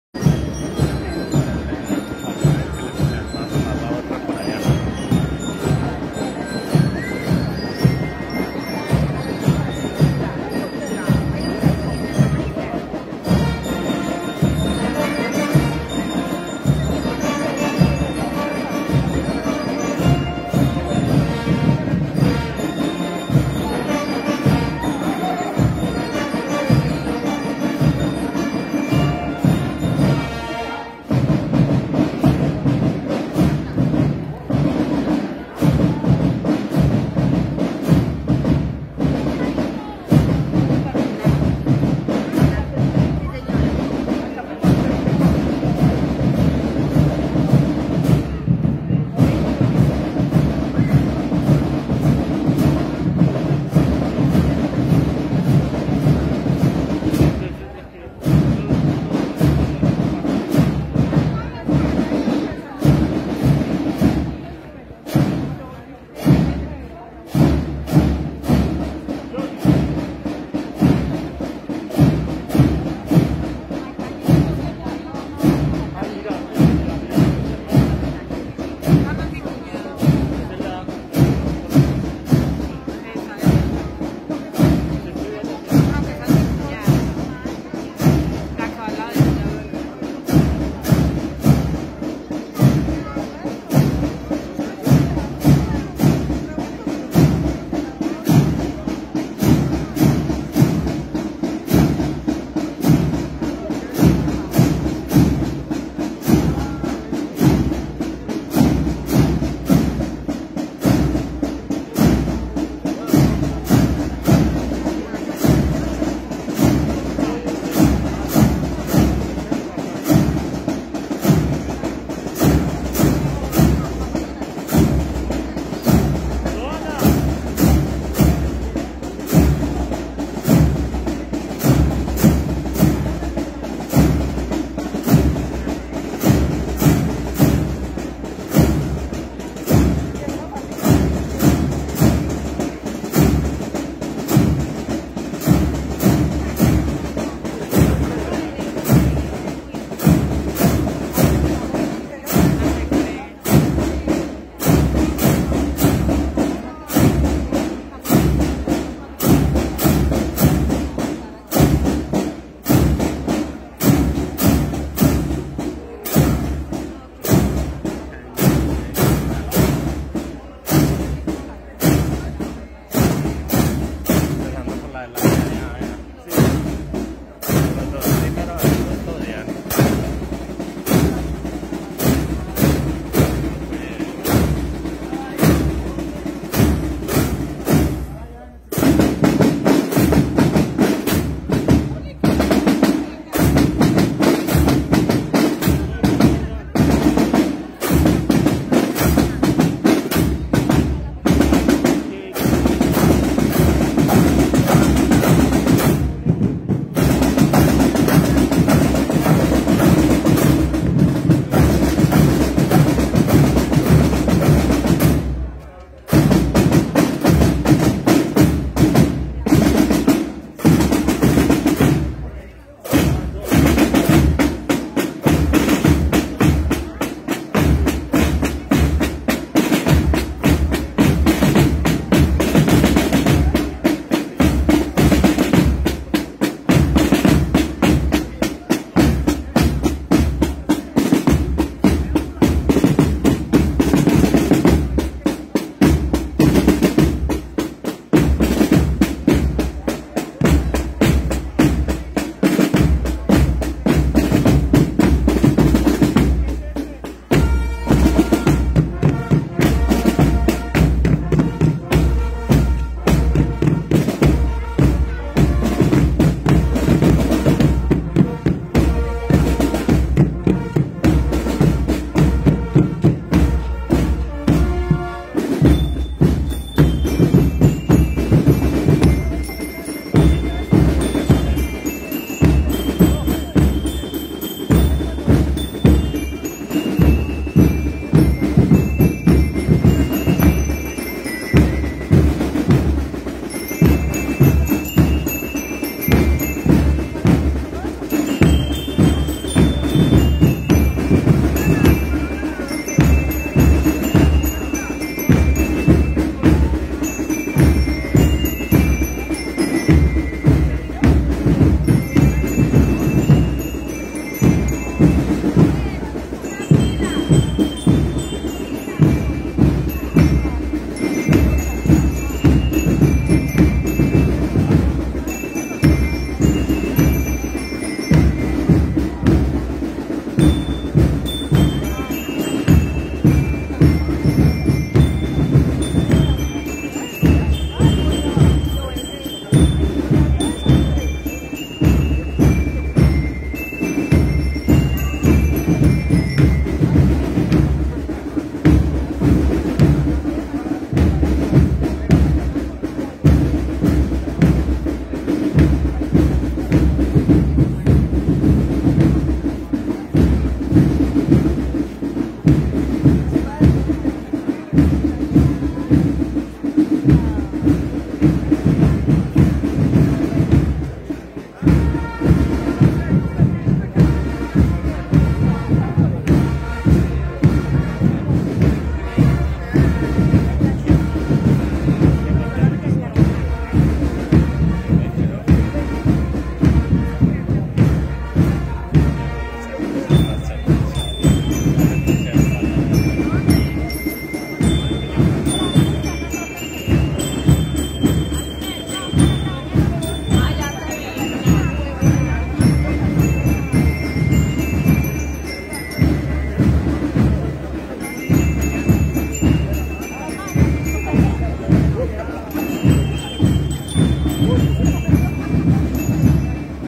Banda Musico Marcial I.E. Cardenal Anibal Muñoz Duque, Santa Rosa de Osos 2022
Festival de bandas Marciales y músico marciales Fiestas del atardecer Santa Rosa de Osos (Antioquia)
Cortesía
LS Darío Vancho